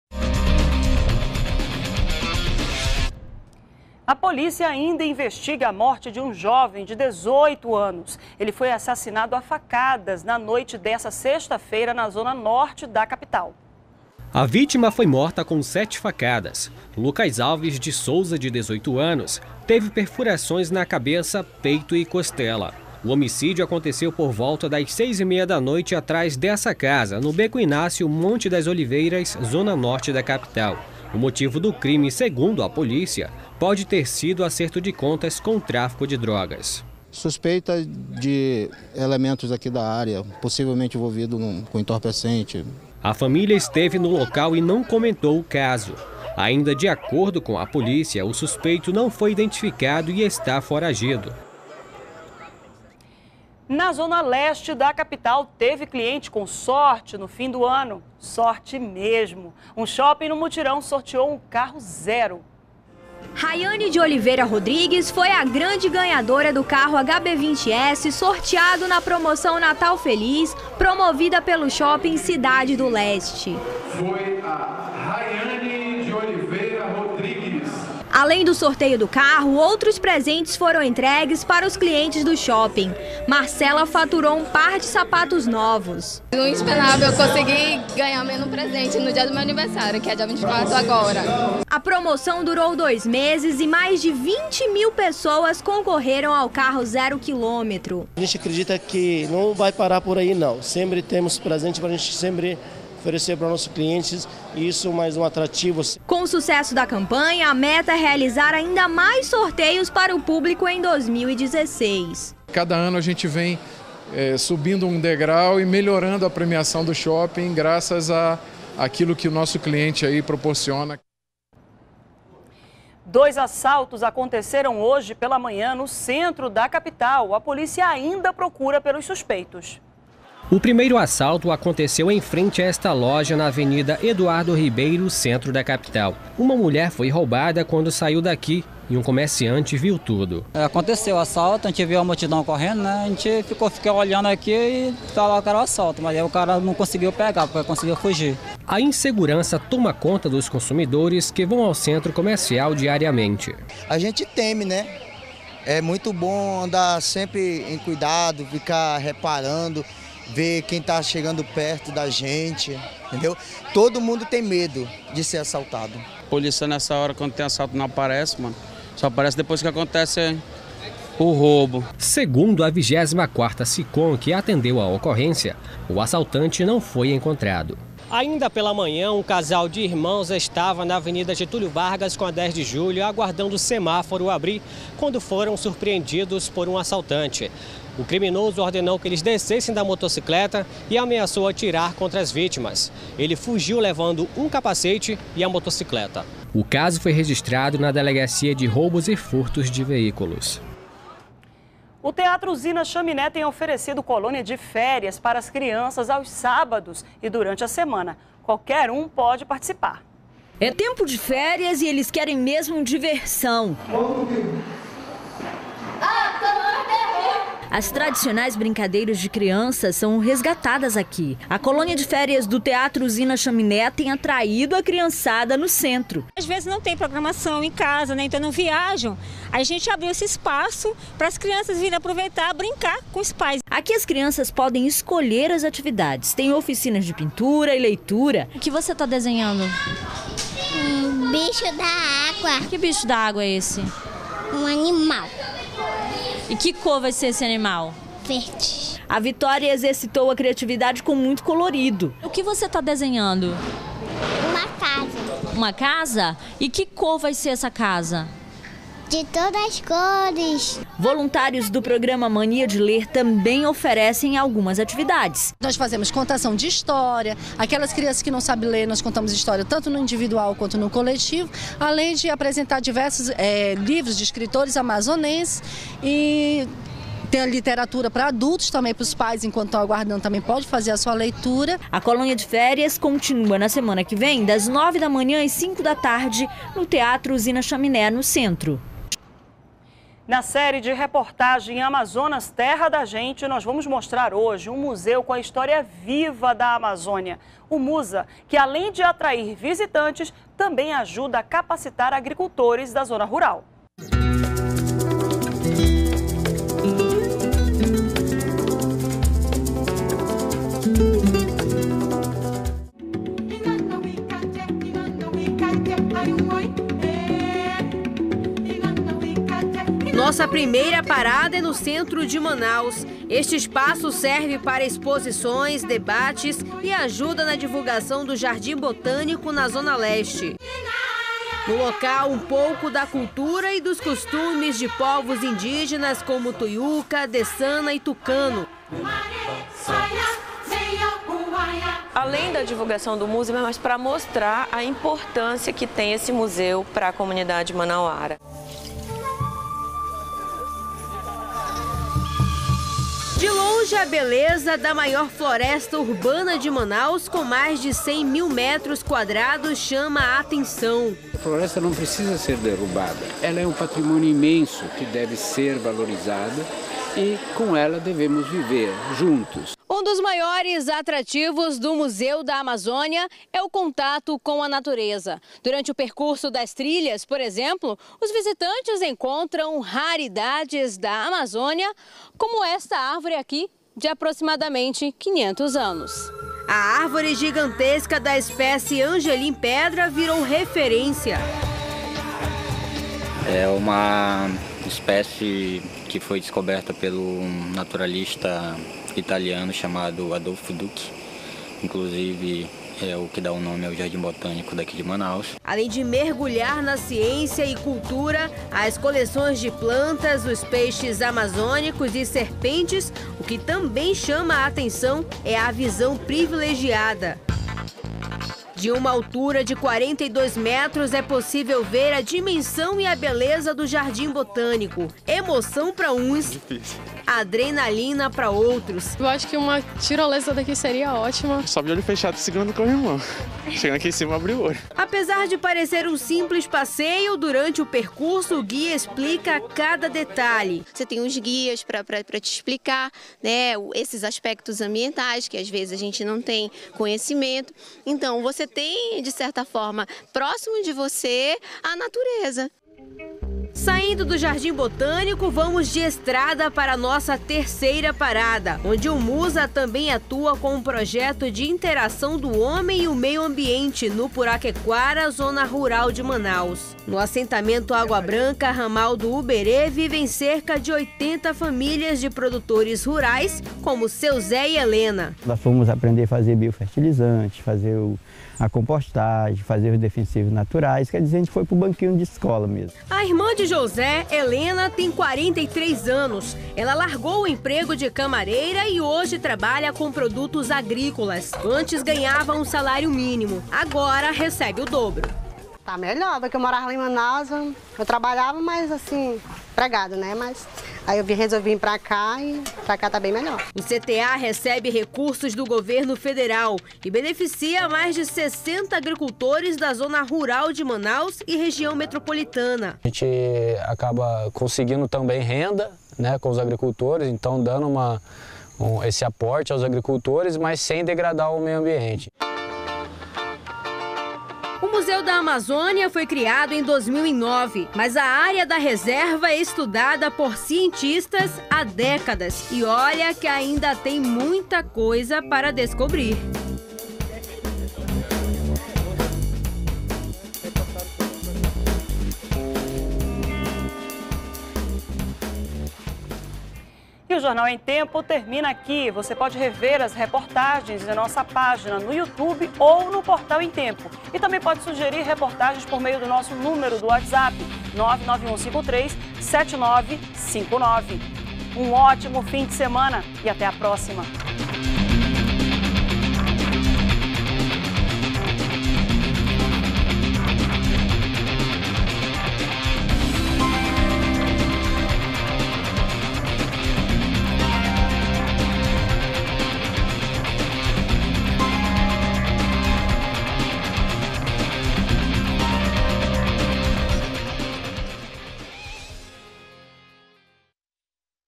Reportagem Amazonas Terra da Gente, mostrar um museu com a históra viva da amazônia
02º BLOCO JORNAL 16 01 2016 - Reportagem Amazonas Terra da Gente, mostrar um museu com a históra viva da amazônia